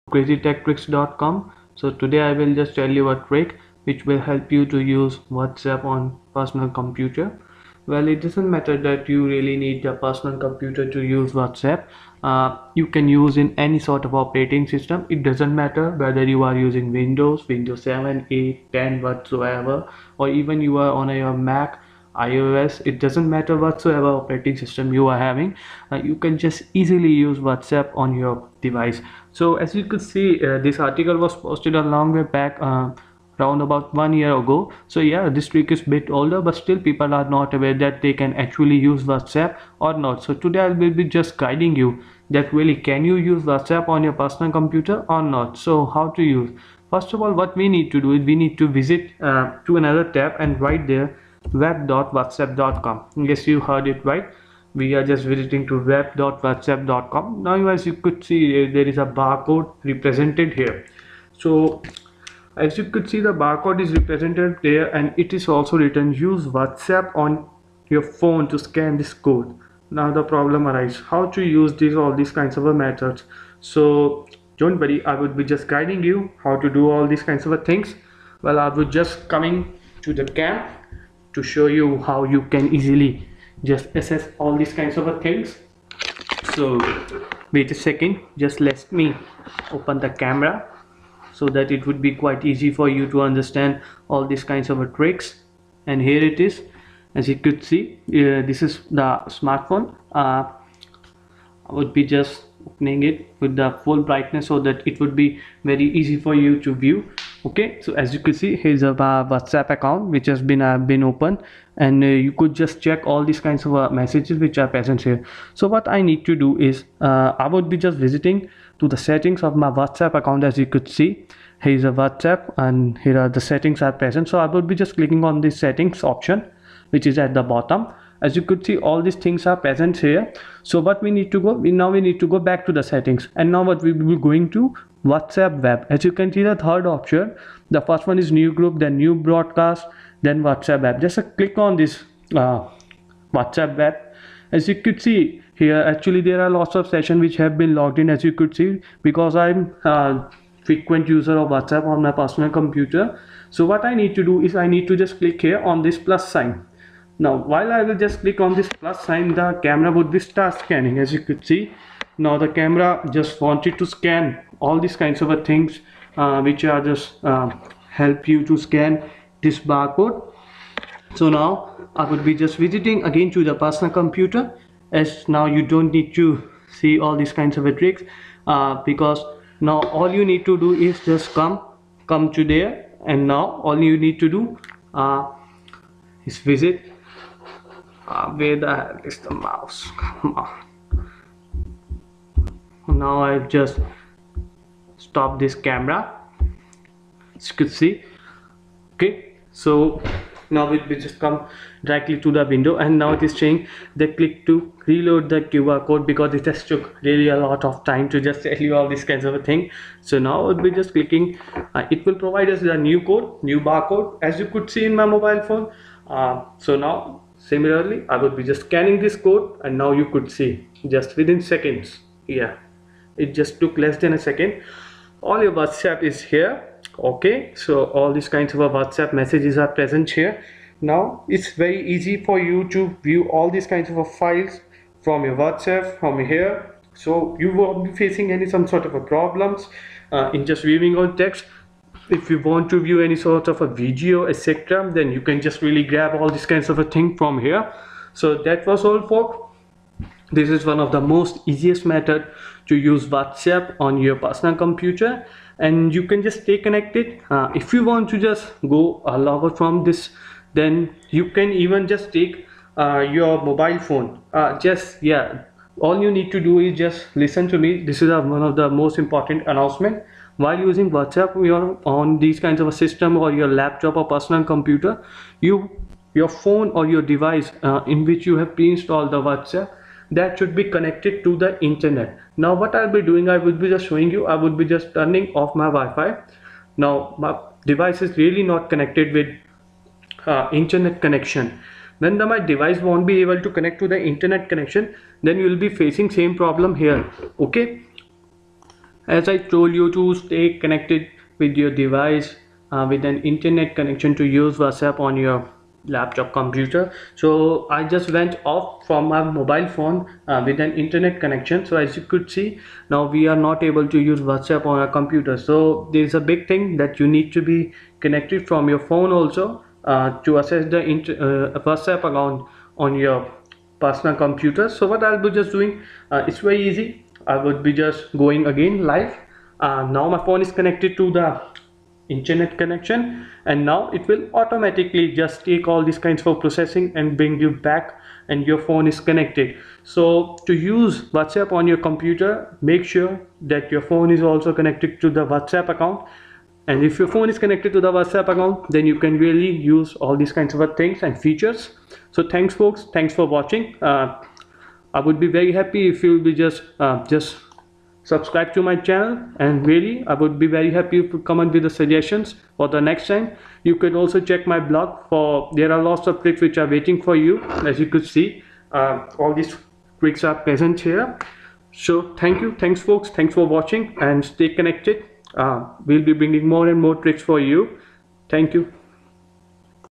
How to use Whatsapp on your computer
Use Whatsapp on your computer, Mac or any operating system.